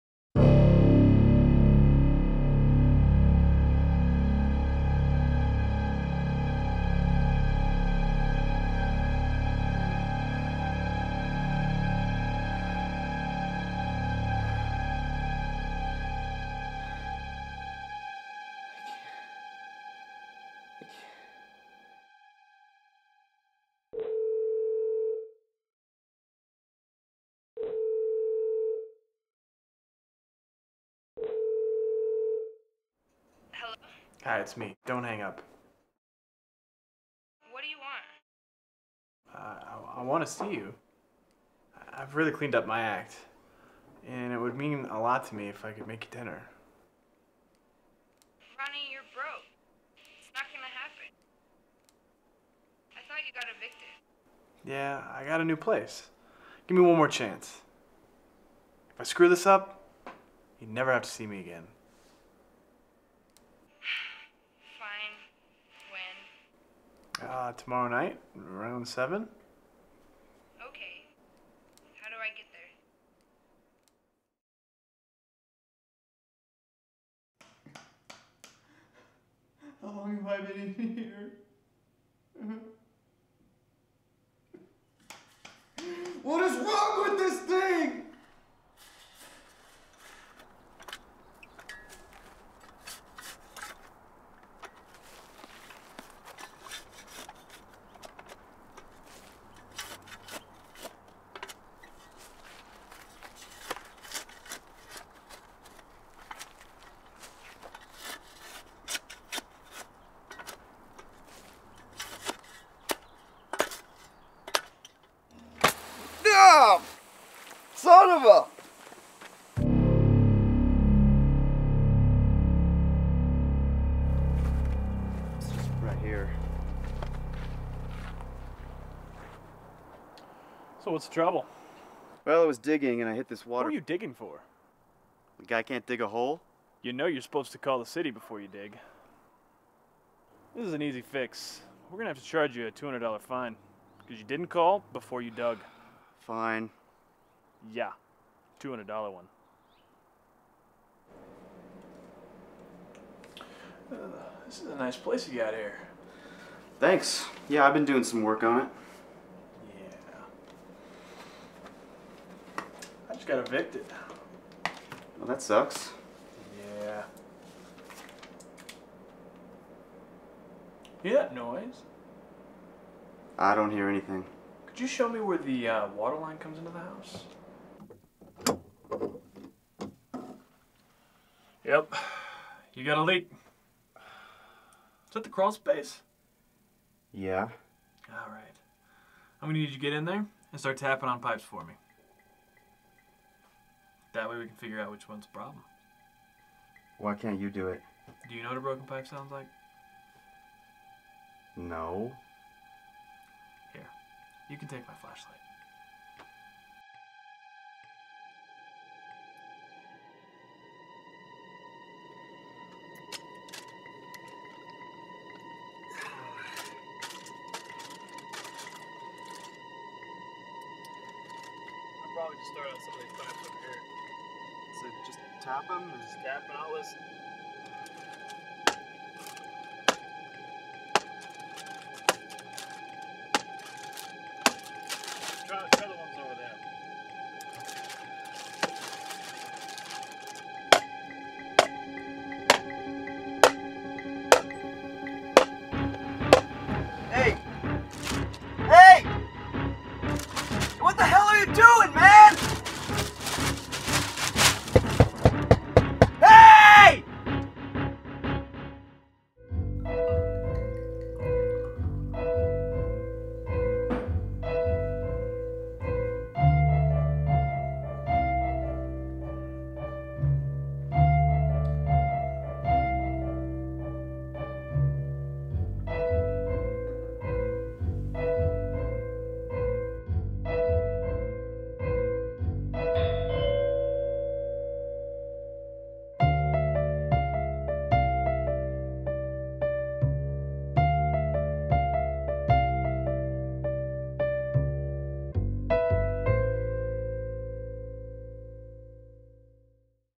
811
Ronnie the plumber takes advantage of a client.